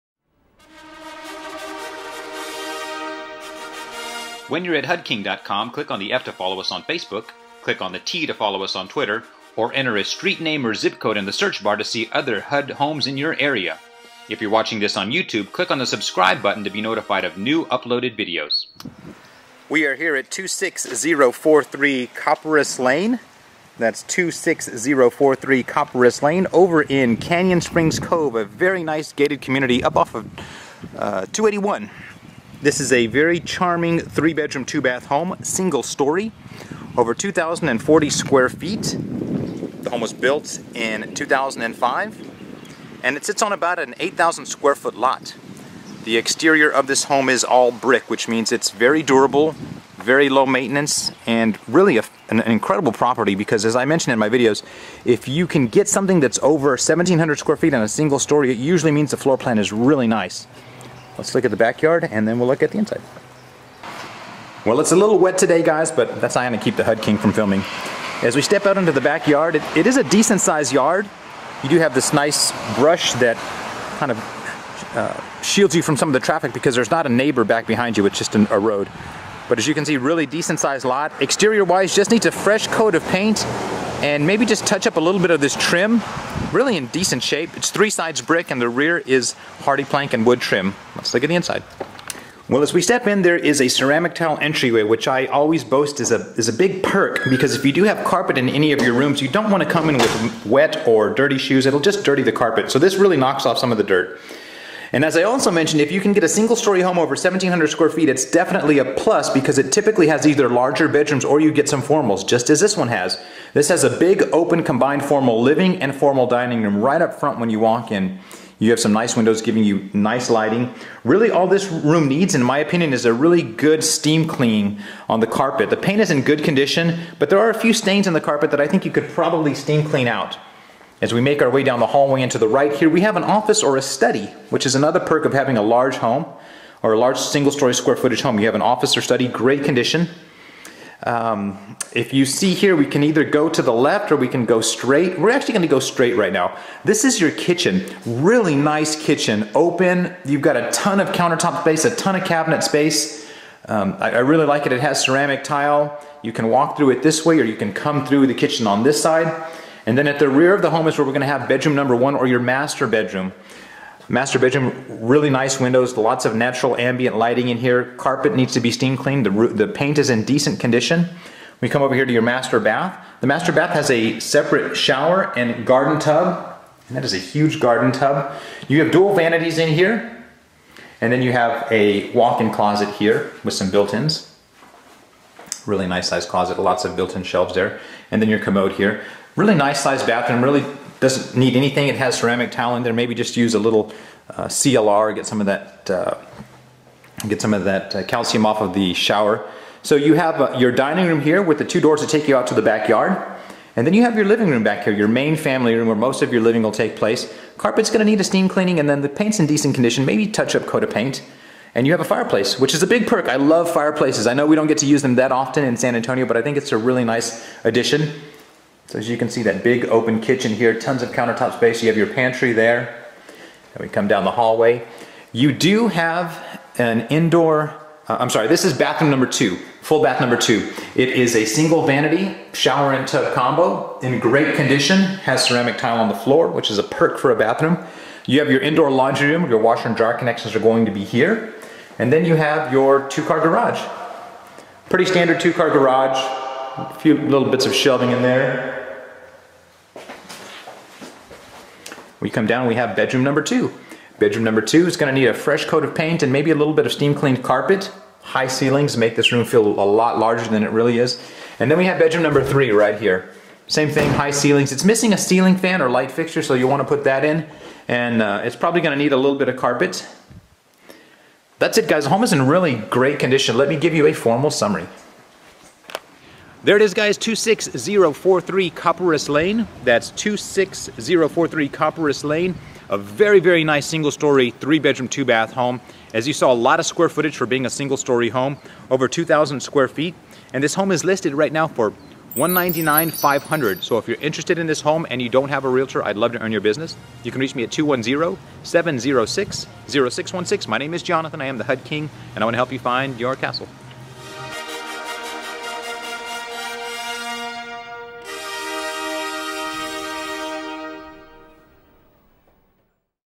San Antonio HUD Homes -- HUD King tours 26043 Copperas Ln.
Is San Antonio Real Estate a good investment? Watch as The HUD King delivers a real virtual tour of the HUD Home located at 26043 Copperas Ln., San Antonio, TX, 78260. This large single story 3 bedroom 2 bath home is nestled quietly in a small gated community. There's certainly pride of ownership in here because all the homes are well manicured and maintained. This home is about 2,046 square feet and was built in 2005 an is almost move in ready. Other than a little touch up paint and flooring, it's pretty much ready to go! This home is listed for $199,500 and HUD will pay 3% of your closing costs.